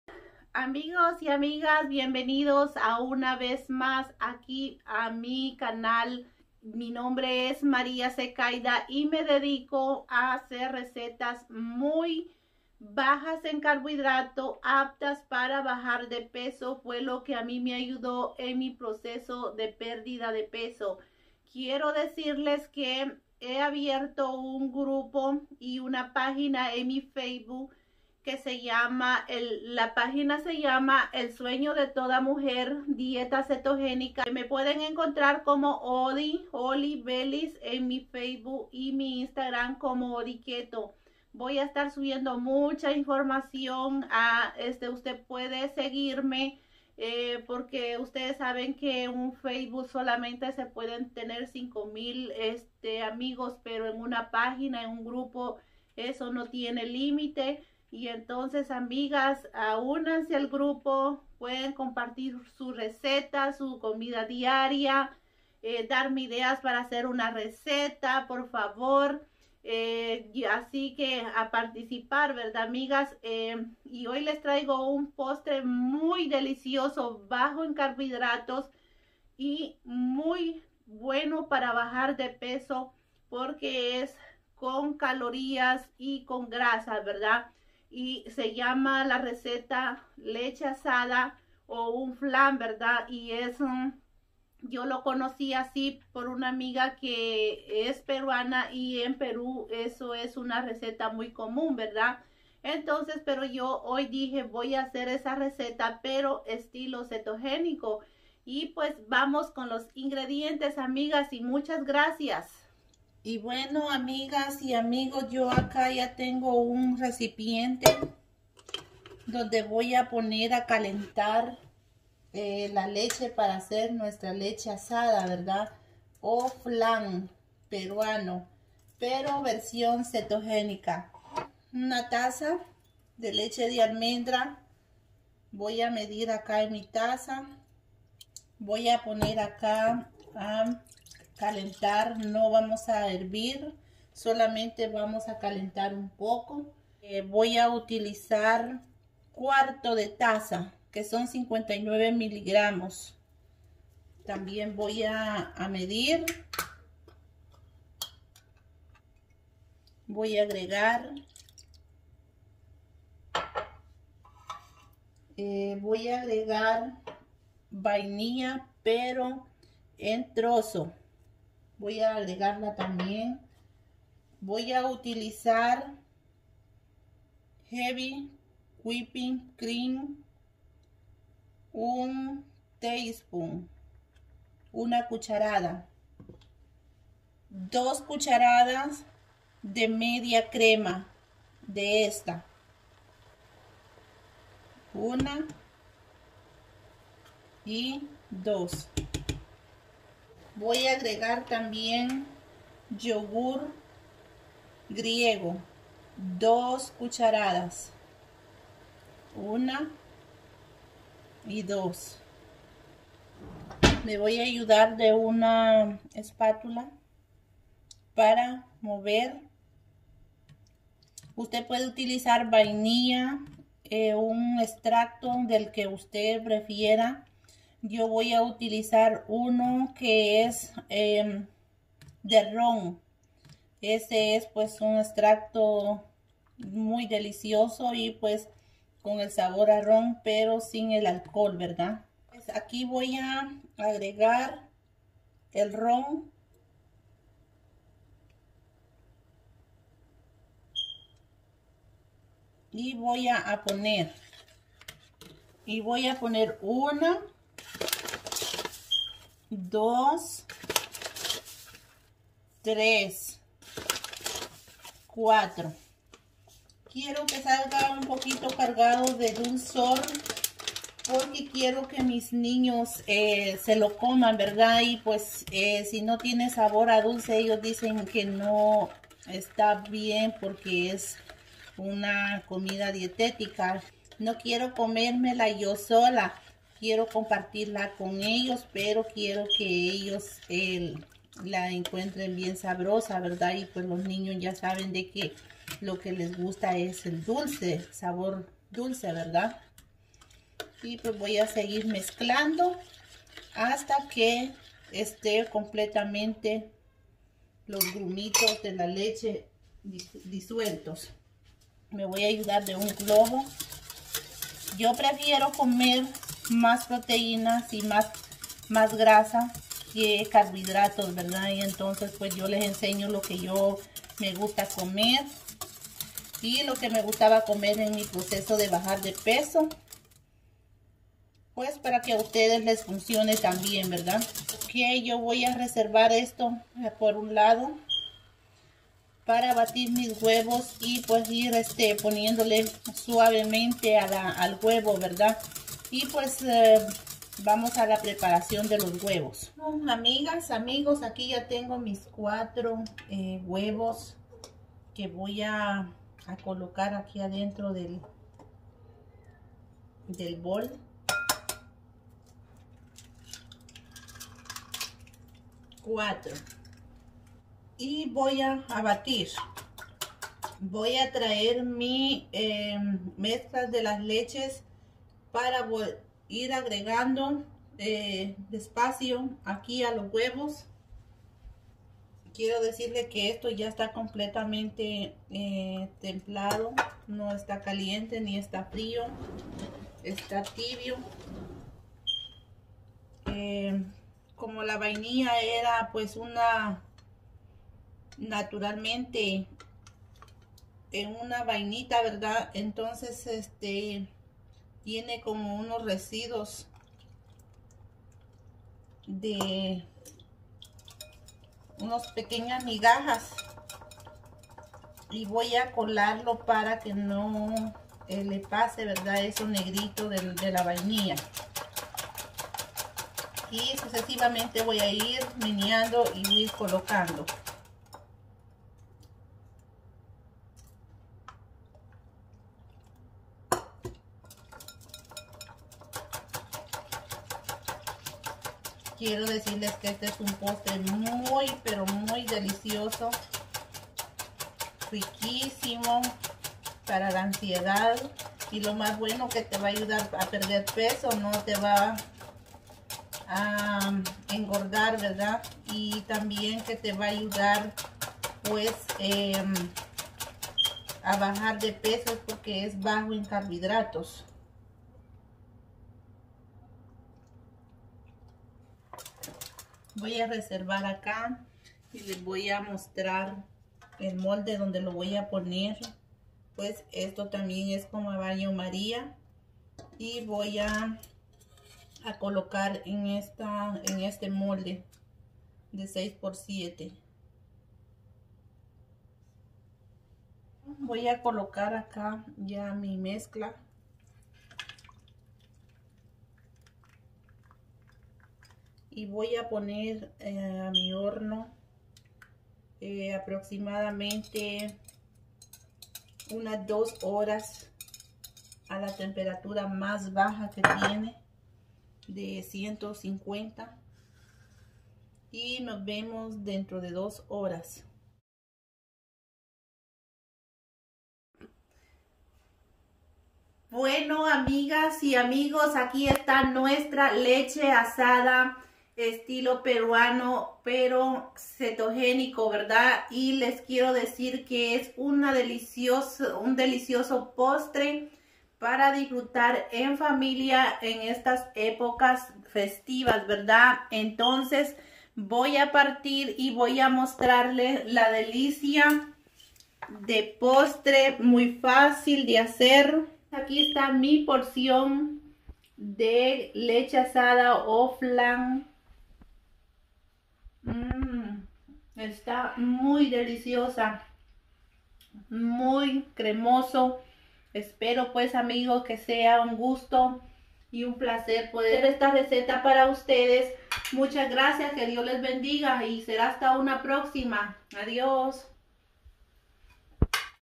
Como Preparar Flan Cetogenico para bajar de peso!🍮😋
Ingredientes:
Leche De Almendras una taza
Media crema 2 cucharadas
4 huevos 🥚
56mm leche de coco
Yogurt griego
Endulzante asu gusto
Vainilla
Rum

150f por 2 horas al horno

Macros:
Carbohidratos 7
Proteína 28
Grasa 24
Calorías 364

Divididas por 7 son 52 calorías
4 proteína 3 de grasa 1 carbohidratos